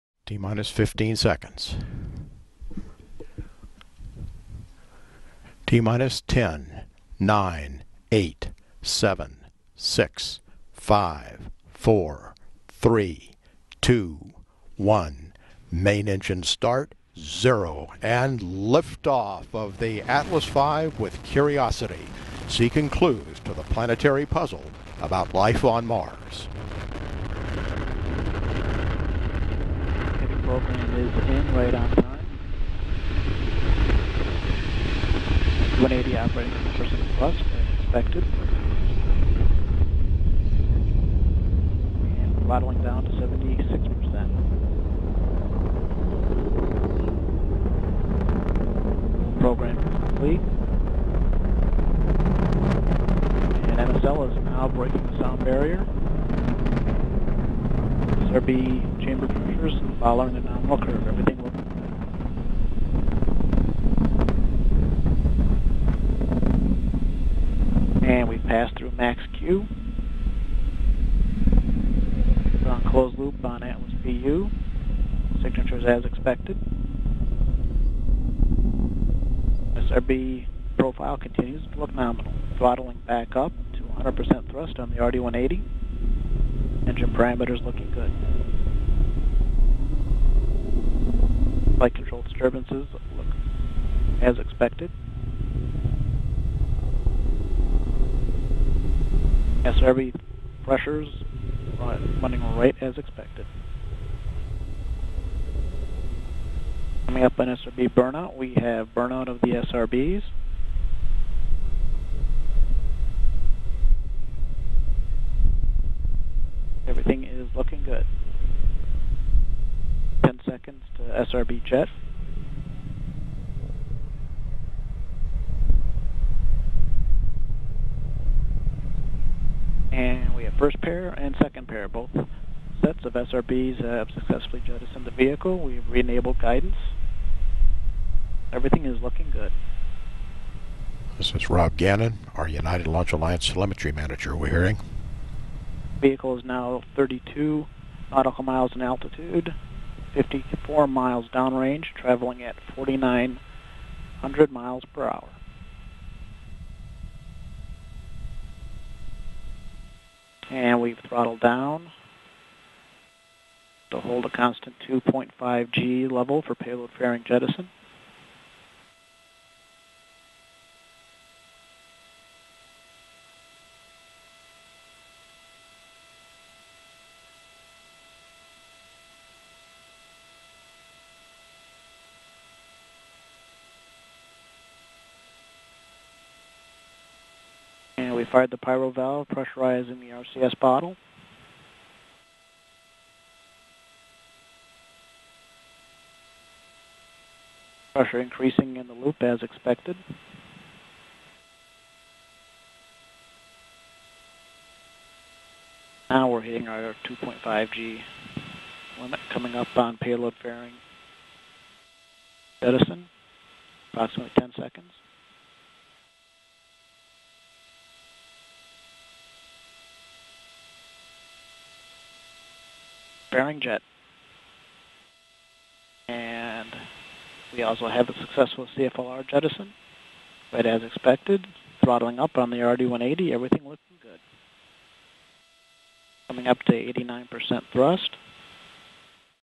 Pocetak istrazivanja Marsa
Pocetak planetarnog istrazivanja.
Mars na dohvat ruke. Za nekih 7 meseci rover ce da bude na Crvenoj planeti...